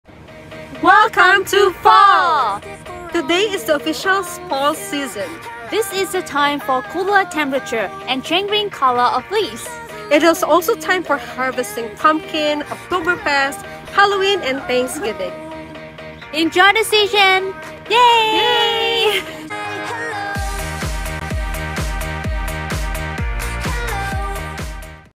Hello Fall 2023!
Hello International Students,

Today marks the first day of Fall! It Is one of the favourite seasons for many of us! This is the time to witness changing green to vibrant colourful leaves, harvesting and carving pumpkins, Oktoberfest, Halloween, and Thanksgiving.

To keep you updated on fall events at your campus, please follow these college accounts below:

Conestoga Student Engagement
CSI Students Inc

Are you excited for the fall season?